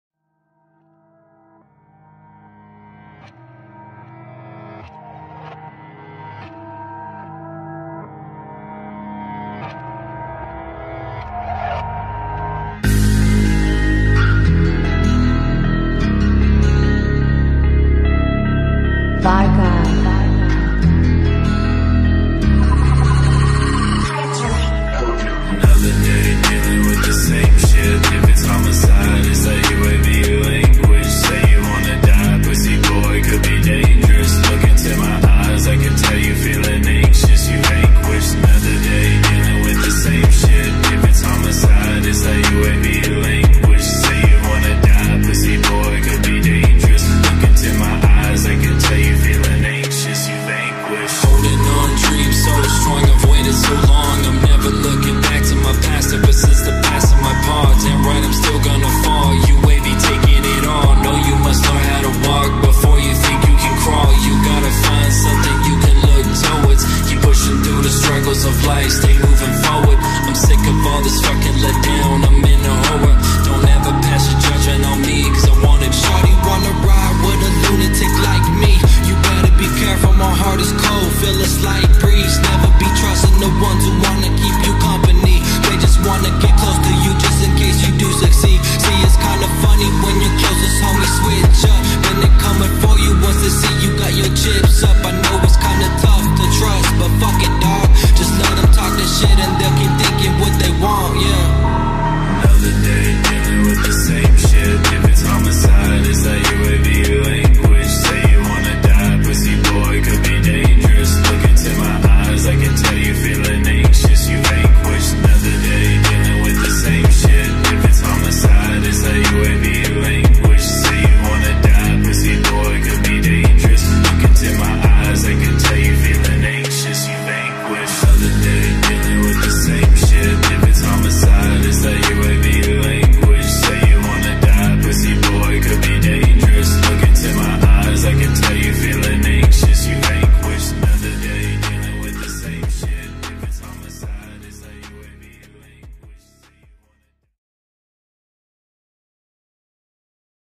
-QuedadaCalistenicos2023halloween-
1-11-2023 en las barras de la playa de Sagunto. mucha calidad y motivación para todos los calistenicos.